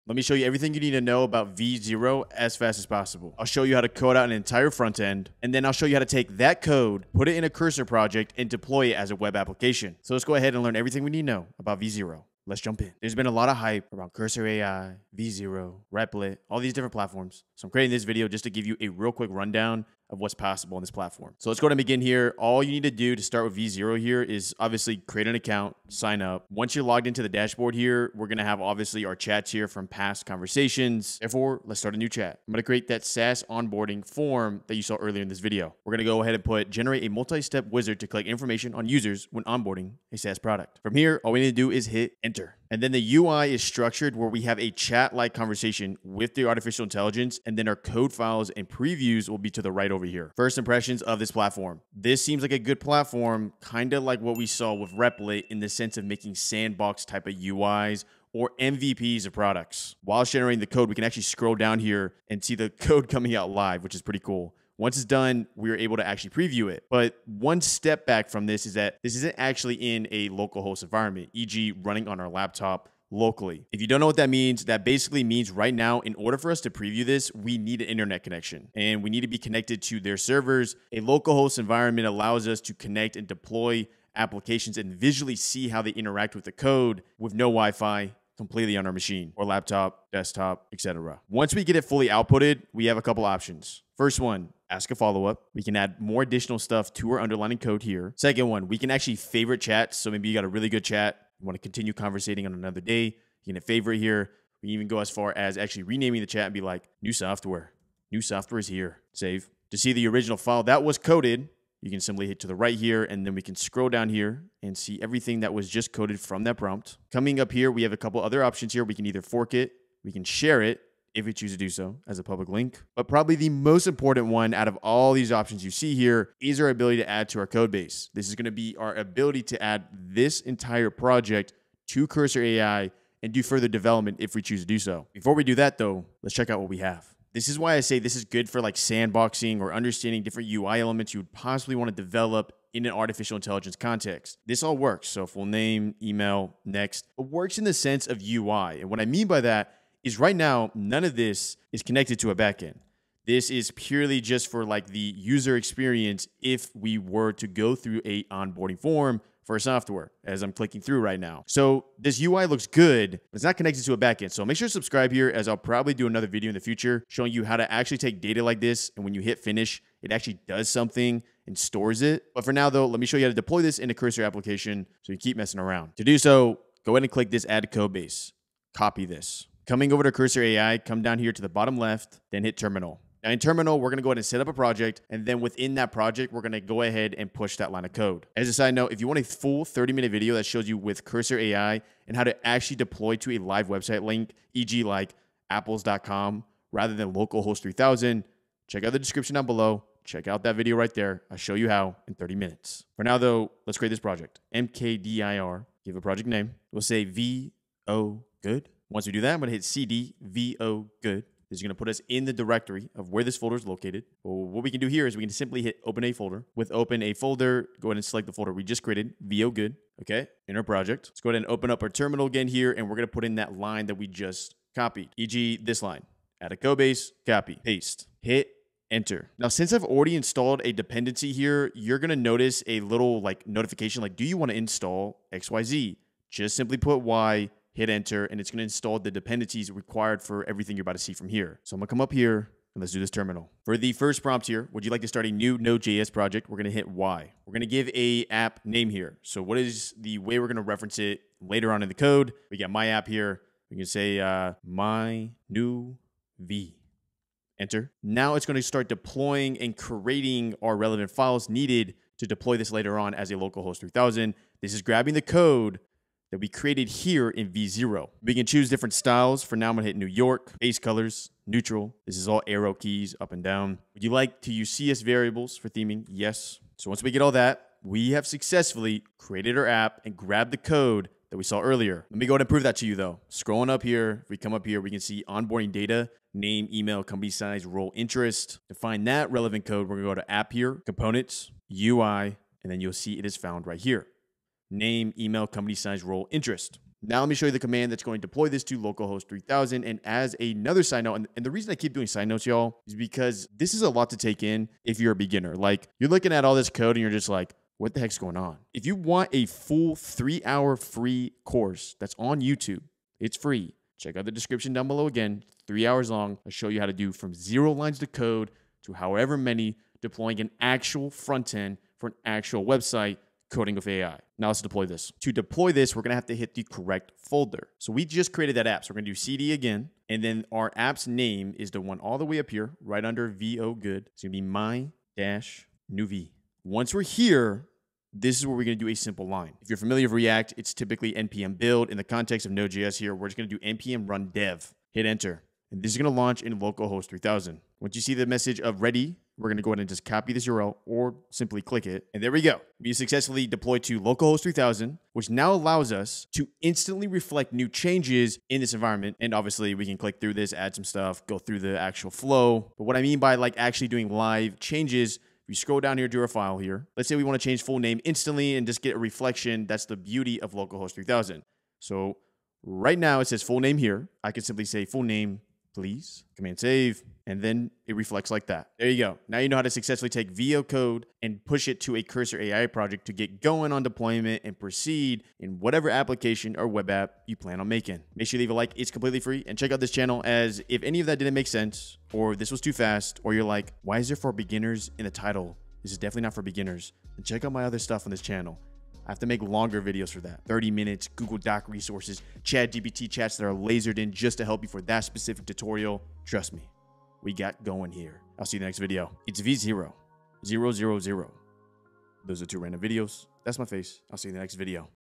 How To Use v0 by Vercel For Beginners
v0 by Vercel is a generative UI tool designed to assist beginners in web development by providing technical guidance, generating user interfaces, and writing code in JavaScript and Python.

v0 by Vercel
Chat with v0. Generate UI with simple text prompts. Copy, paste, ship.

Key Takeaways:

✩ v0 generates code, UI components, and responses based on simple text prompts.
✩ It supports multiple frameworks and libraries, including React, Svelte, Vue, Bootstrap, and Material UI.
✩ Generate UI with simple text prompts. Copy, paste, ship.

How to build: a v0.dev clone (Next.js, GPT4 & CopilotKit)

➤ other places I exist